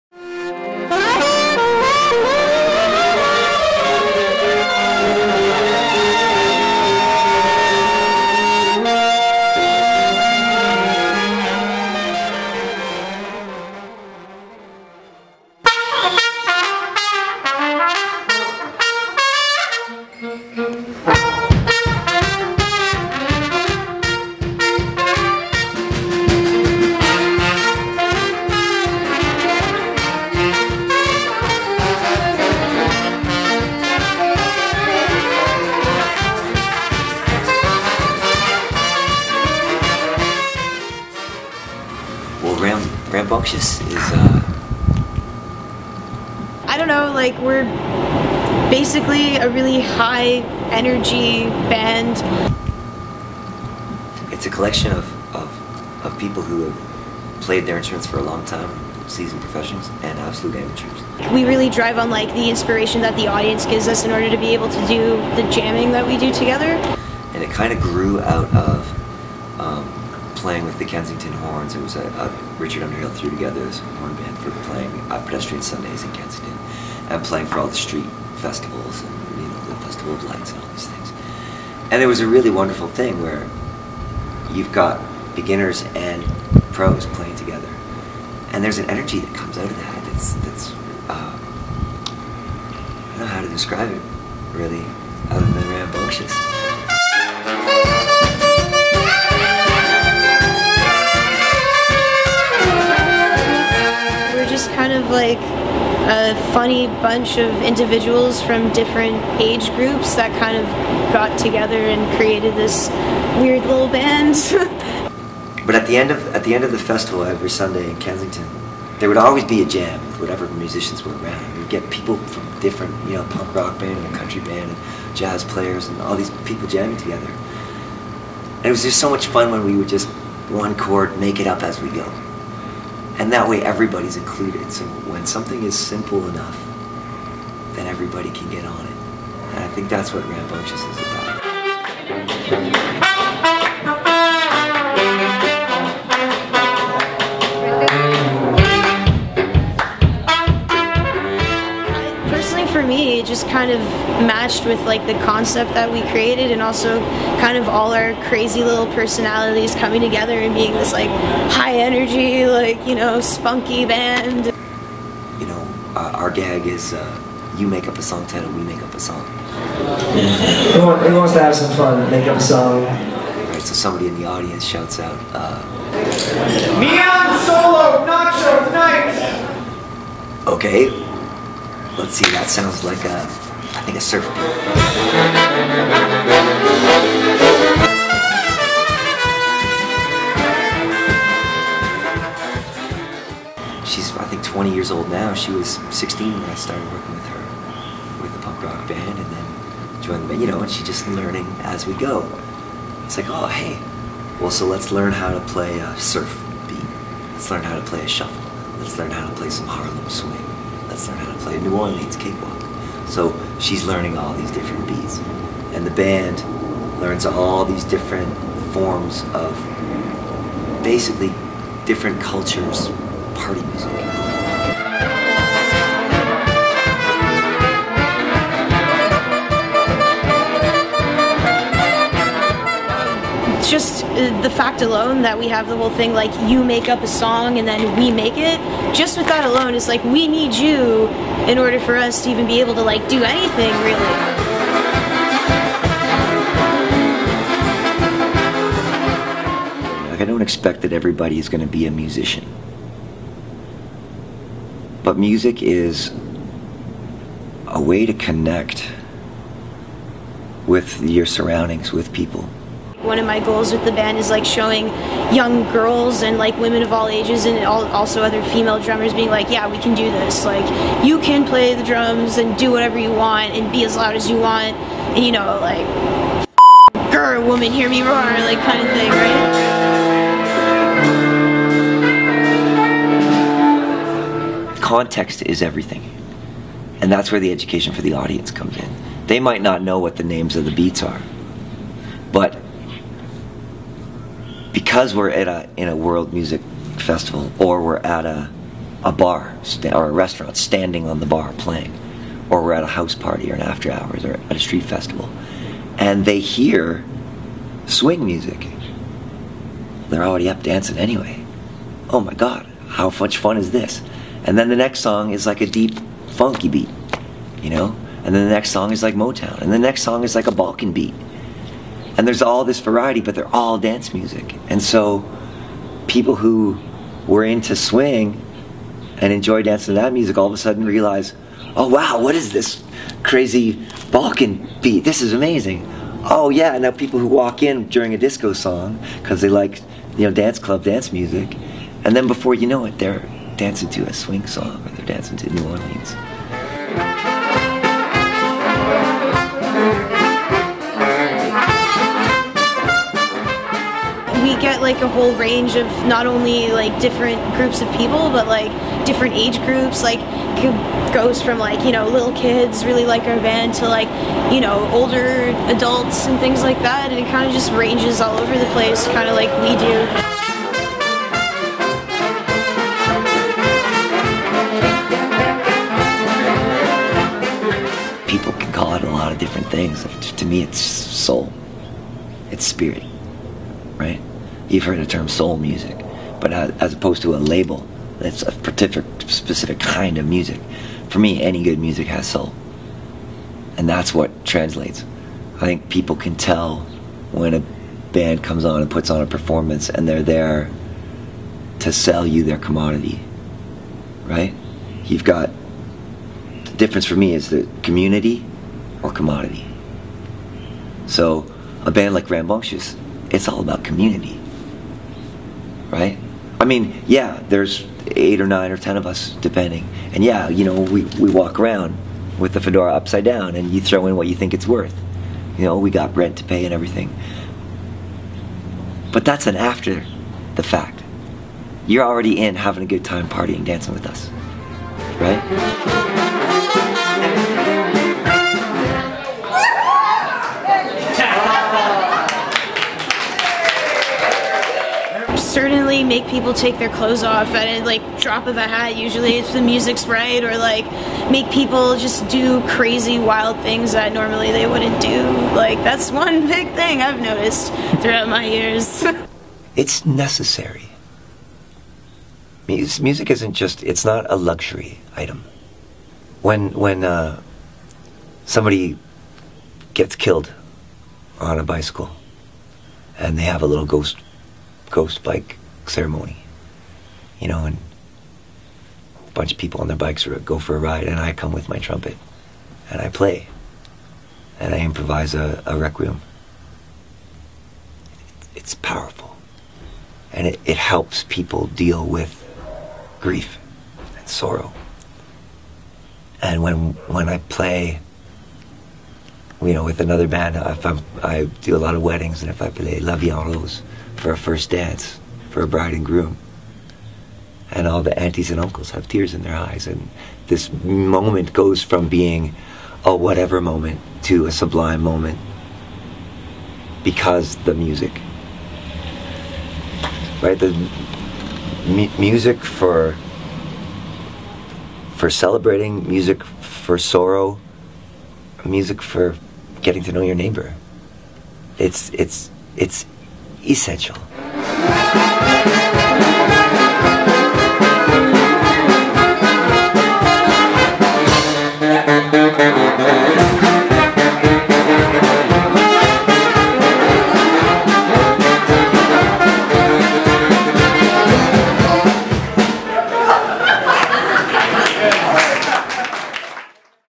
Voice of World Music Today - Rambunctious
Small World Music presents Rambuntious @ Lula Lounge on September 20, 2012 .

Toronto, Canada
Improvised World Dance
ram-bunc-tious (adj)
Noisy, very active, and hard to control, usually as a result of excitement or youthful energy.
Nine horns + one drummer = dance party. You make a title, we make a song.
Toronto rabble-rouser Michael Louis Johnson leads jams of swing, funk, balkan-beat and more.